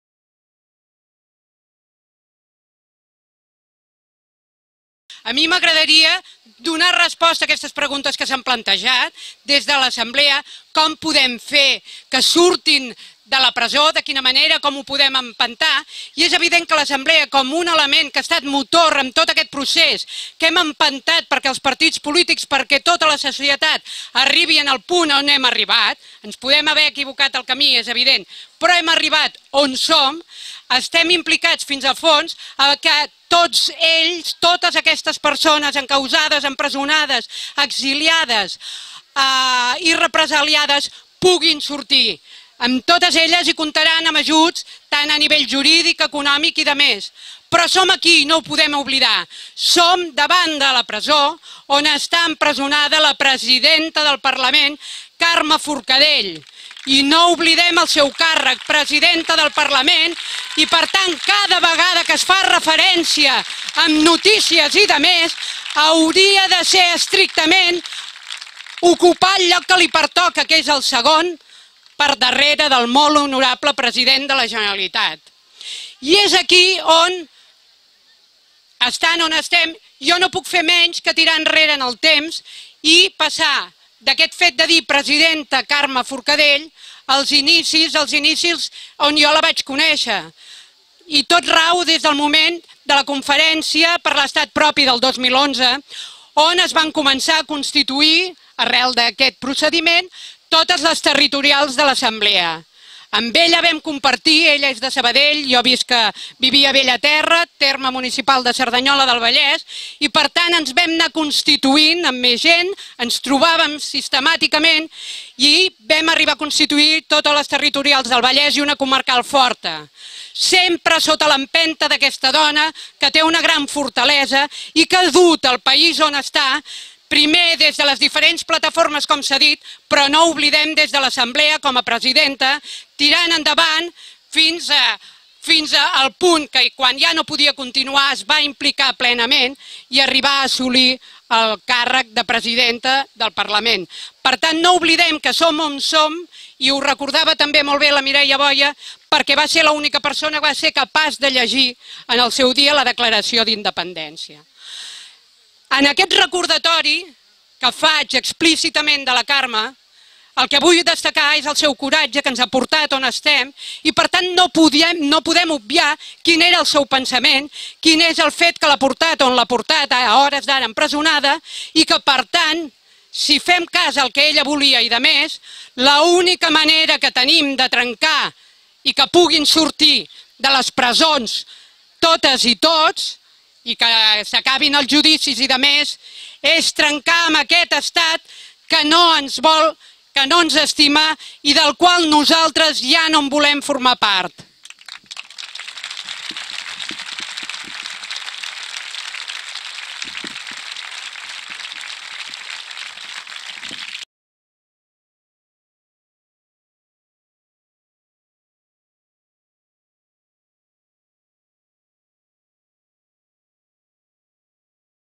Eulalia Subirats. Acte Unitari en suport a MHP presidenta Carme Forcadell a Mas d'Enric. 23 08 2018
Milers de persones es concentren en suport de Carme Forcadell a Mas d'Enric, que avui compleix el seu cinquè mes en una presó espanyola perquè va permetre un debat al Parlament de Catalunya.

Això és el que fa que la República catalana sigui imparable, que ha mobilitzat gent jove i gent gran, gent de dretes i d’esquerres, gent amb els avis catalans i gent nouvinguda, gent de tots els estrats socials,... Tot un poble tossudament i pacíficament alçat!

"A sota d’aquest cos que fermes ara,
què creus que hauràs fermat a la presó?
 Mac Sweney lliure, i més encès encara,
vola al davant dels seus i els dóna ardor."

(versos de Ventura Gassol)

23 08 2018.